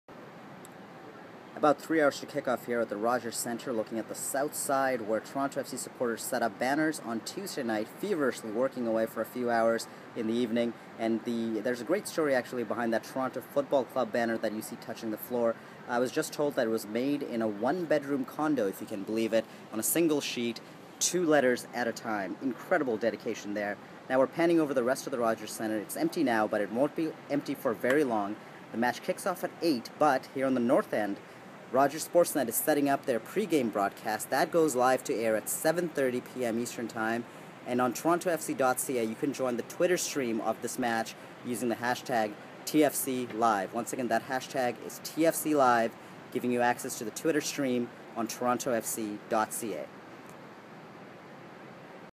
Three Hours To Kickoff
Toronto FC takes on LA Galaxy in CCL quarterfinals on Wednesday. Watch live at 8 p.m. ET on Rogers Sportsnet and use TFCLive on Twitter to join the discussion at TorontoFC.ca.